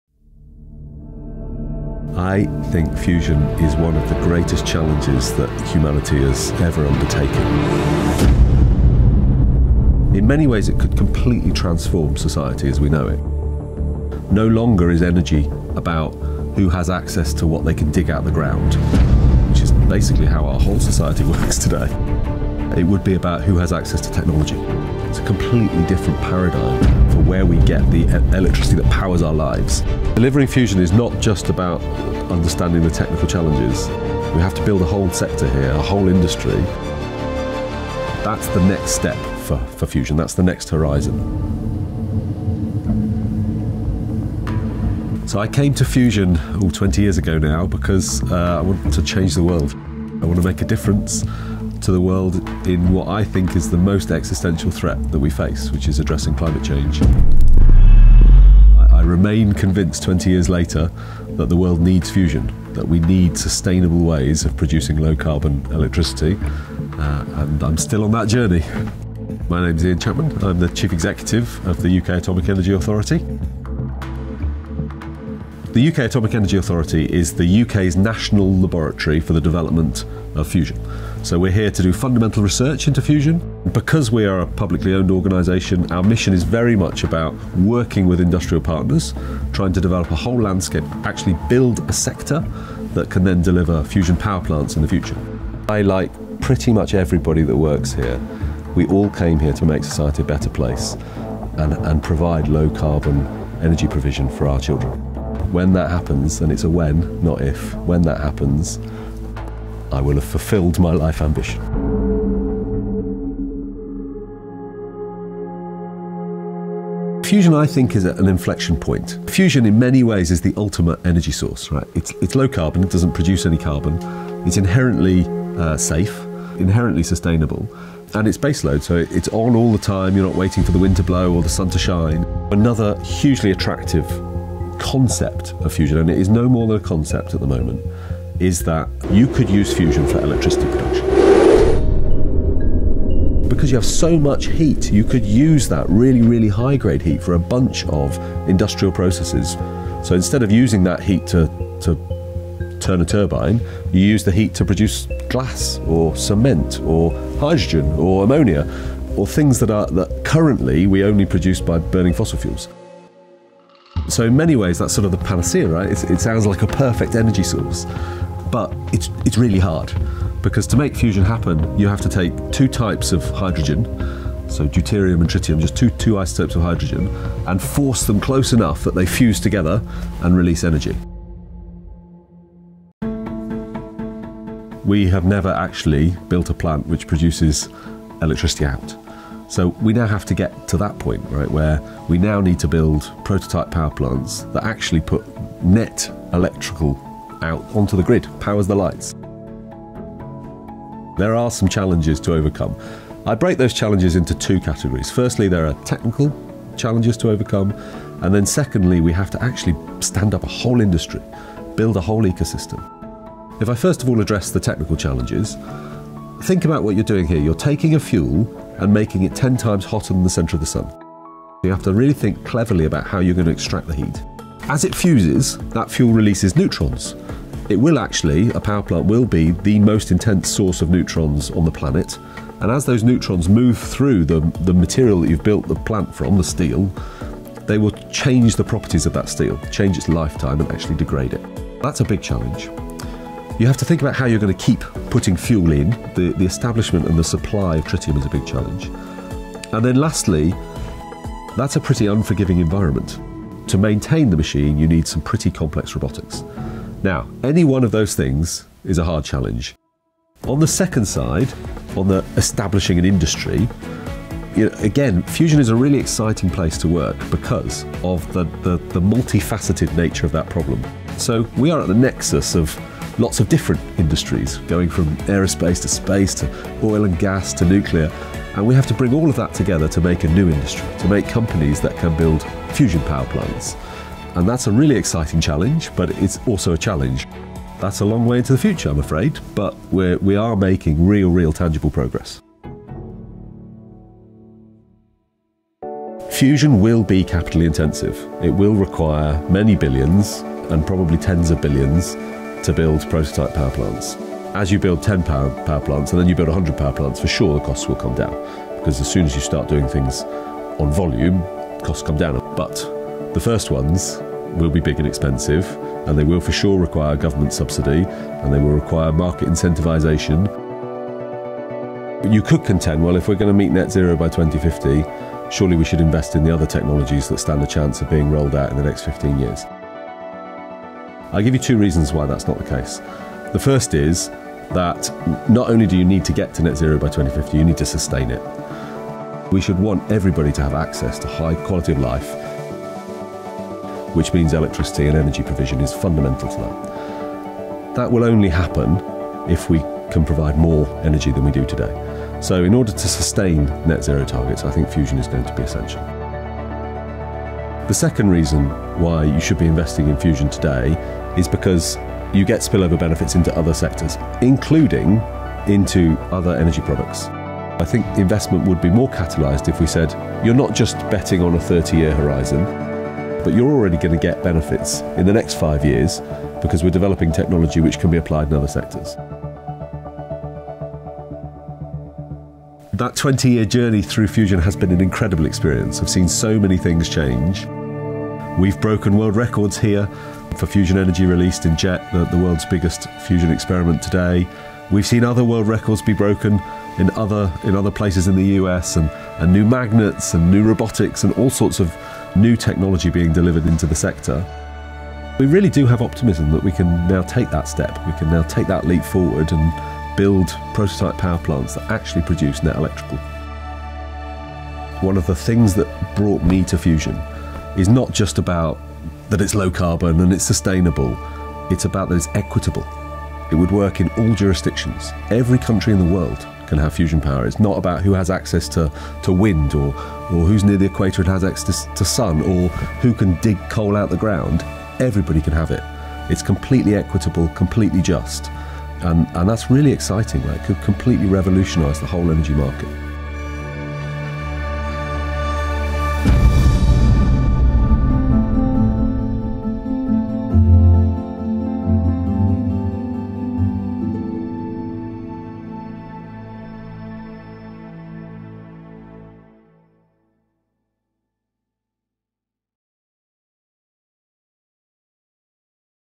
Is Fusion Energy The Gamechanger Our Planet Needs?
Fusion could be the ultimate source of low carbon energy, but achieving the process on Earth is no small feat. What will it take to unlock its immense potential?

As Sir Ian Chapman explains, technology is essential to success, but it's not the only factor. To deliver fusion and scale its impact, we must also build a new sector from the ground up.

Sir Ian is CEO at UK Atomic Energy Authority – the country's national laboratory for the development of fusion. UKAEA's mission is to lead the delivery of sustainable fusion energy while also maximising its scientific and economic benefits.

By undertaking fundamental research and working with industrial partners today, they aim to create a landscape that can support fusion power plants in the future.

Fusion is currently at an inflexion point. It's inherently safe, sustainable and can replace fossil fuels as a source of baseload energy, all of which adds to its appeal. What's stopping us from reaping the benefits? According to Sir Ian, the process itself poses difficulties.

To achieve fusion two atomic nuclei must be fused together. The collision releases energy, but also requires it in staggering amounts. We are yet to build a plant that produces more energy than it uses.

The obstacles are real, but according to Sir Ian, not insurmountable. Technology and industry both have a pivotal role to play. Intensive capital is needed to scale, but with the world of opportunity it represents, fusion is a challenge worth investing in.